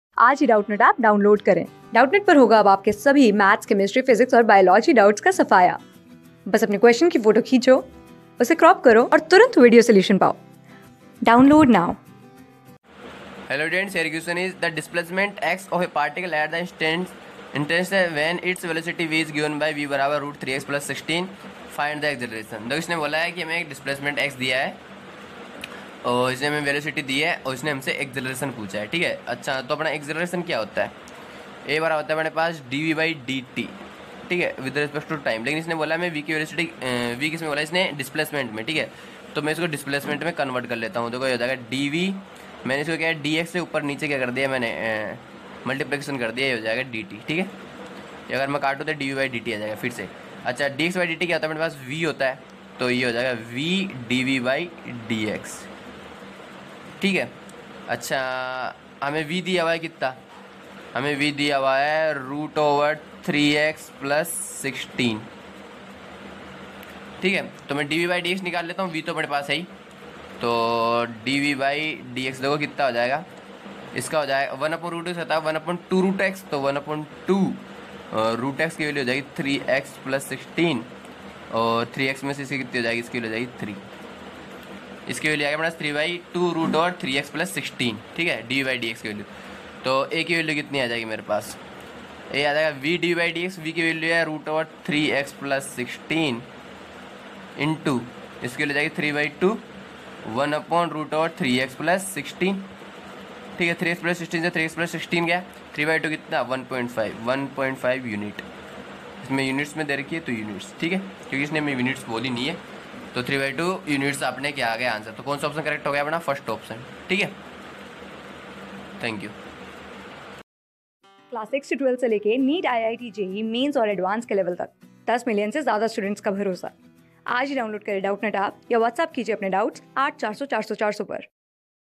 [ The displacement x of a particle at the ],[ instant when its velocity v is given by ],[v=sqrt(...
[ The displacement x of a particle at the ],[ instant when its velocity v is given by ],[v=sqrt(3x+16) .Find its acceleration ],[[ (A) 1.5 units ],[ (B) 2.5 units ],[ (C) 3.5 units ],[ (D) 4.5 units ]]
Class: 12
Subject: PHYSICS
Chapter: RECTILINEAR MOTION
Board:USER_ASKED

👉 Have Any Query? Ask Us.